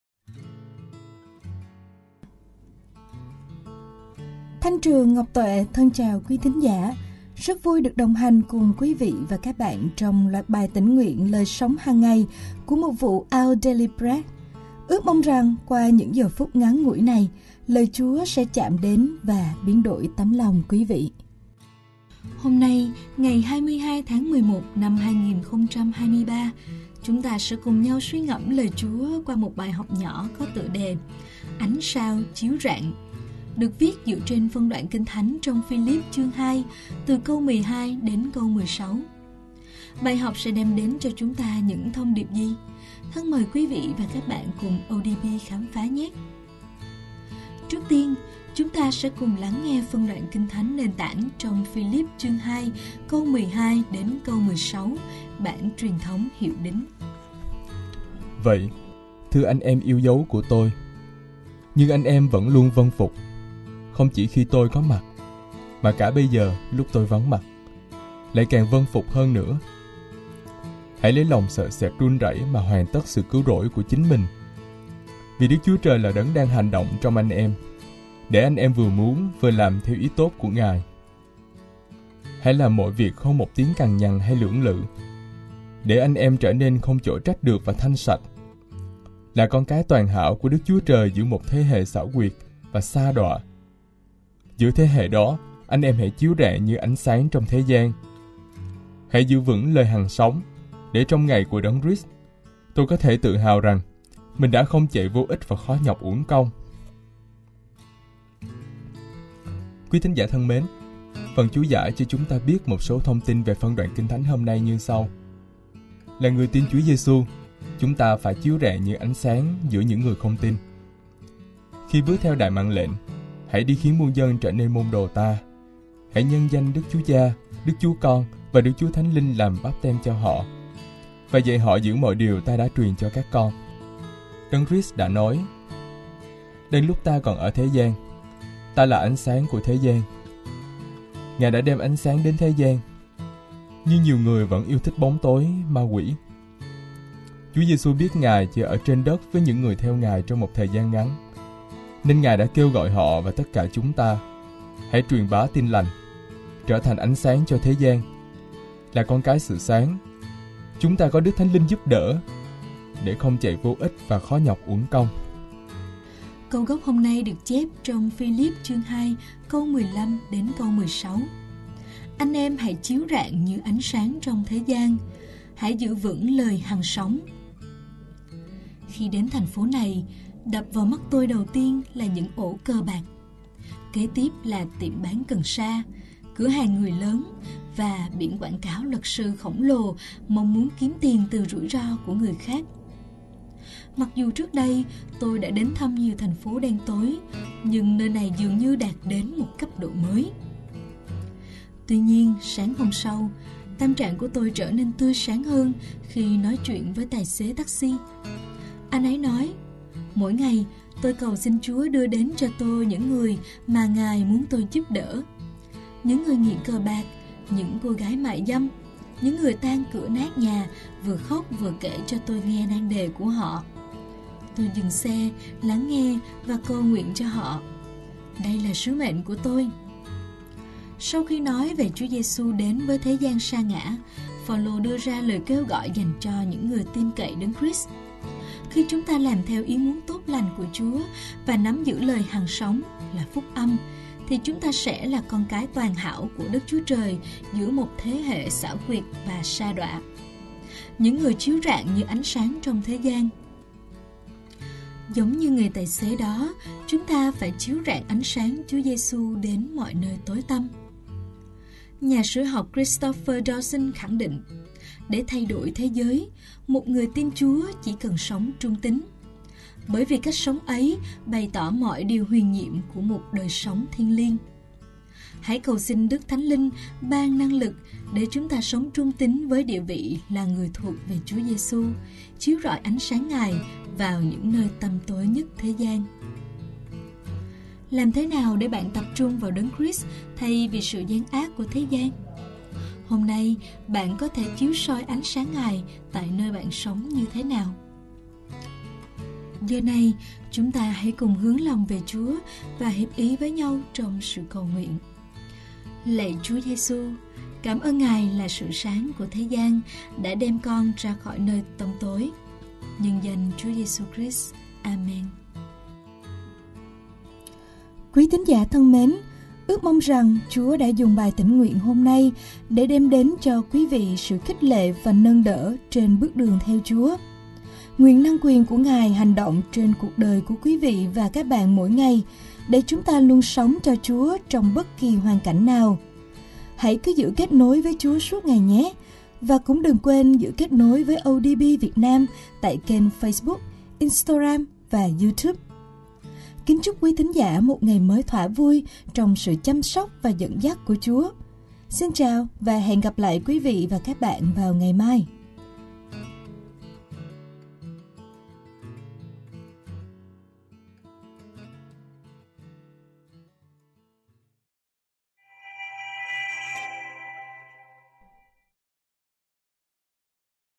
Ánh Sao Chiếu Rạng | Tĩnh nguyện Lời Sống Hằng Ngày | 20 Tháng 11, 2023
Khi đến thành phố này, đập vào mắt tôi đầu tiên là những ổ cờ bạc. Kế tiếp là tiệm bán cần sa, cửa hàng “người lớn”, và biển quảng cáo luật sư khổng lồ mong muốn kiếm tiền từ rủi ro của người khác. Mặc dù trước đây tôi đã đến thăm nhiều thành phố đen tối, nhưng nơi này dường như đạt đến một cấp độ mới.

Tuy nhiên sáng hôm sau, tâm trạng của tôi trở nên tươi sáng hơn khi nói chuyện với tài xế taxi. Anh ấy nói: “Mỗi ngày, tôi cầu xin Chúa đưa đến cho tôi những người mà Ngài muốn tôi giúp đỡ. Những người nghiện cờ bạc, những cô gái mại dâm, những người tan cửa nát nhà vừa khóc vừa kể cho tôi nghe nan đề của họ. Tôi dừng xe, lắng nghe và cầu nguyện cho họ. Đây là sứ mệnh của tôi.”

Sau khi nói về việc Chúa Jêsus đến với thế giới sa ngã (Phil. 2:5–8), Phao-lô đưa ra lời kêu gọi dành cho những người tin cậy Đấng Christ. Khi chúng ta làm theo ý muốn tốt lành của Chúa (c.13) và nắm giữ “lời hằng sống”, là Phúc Âm (c.16) thì chúng ta sẽ là “con cái toàn hảo của Đức Chúa Trời giữa một thế hệ xảo quyệt và sa đọa”, những người “chiếu rạng như ánh sáng trong thế gian” (c.15). Giống như người tài xế đó, chúng ta phải chiếu rạng ánh sáng Chúa Jêsus đến mọi nơi tối tăm.

Nhà sử học Christopher Dawson khẳng định: để thay đổi thế giới, một người tin Chúa chỉ cần sống trung tín; bởi vì cách sống ấy “bày tỏ mọi điều huyền nhiệm của một đời sống thiêng liêng”. Hãy cầu xin Thánh Linh ban năng lực để chúng ta sống trung tín với địa vị là người thuộc về Chúa Jêsus, chiếu rọi ánh sáng Ngài vào những nơi tăm tối nhất thế gian.

Làm thế nào để bạn tập trung vào Đấng Christ thay vì sự gian ác của thế gian? Hôm nay, bạn có thể chiếu soi ánh sáng Ngài tại nơi bạn sống như thế nào?

Lạy Cha kính yêu, con muốn được sống mỗi ngày như một người con quý báu và đáng chuộng trong mắt Ngài.

CHÚ GIẢI
Là người tin Chúa Jêsus, chúng ta phải “chiếu rạng như ánh sáng [giữa những người không tin]” (Phil. 2:15) khi bước theo Đại Mạng Lệnh: “Hãy đi khiến muôn dân trở nên môn đồ Ta, hãy nhân danh Đức Chúa Cha, Đức Chúa Con và Đức Thánh Linh làm báp-têm cho họ và dạy họ giữ mọi điều Ta đã truyền cho các con” (Mat. 28:19–20). Đấng Christ đã nói: “Đang lúc Ta còn ở thế gian, Ta là ánh sáng của thế gian” (Gi. 9:5). Ngài đã đem ánh sáng đến thế gian, nhưng nhiều người vẫn yêu thích bóng tối (ma quỷ) (3:19). Chúa Jêsus biết rằng Ngài chỉ ở trên đất với những người theo Ngài trong một thời gian ngắn, nên Ngài đã kêu gọi họ (và tất cả chúng ta) hãy truyền bá Tin Lành - trở thành “ánh sáng cho thế gian” (Mat. 5:14). Là “con cái sự sáng” (Lu. 16:8), chúng ta có Đức Thánh Linh giúp đỡ để “không chạy vô ích và khó nhọc uổng công” (Phil. 2:16).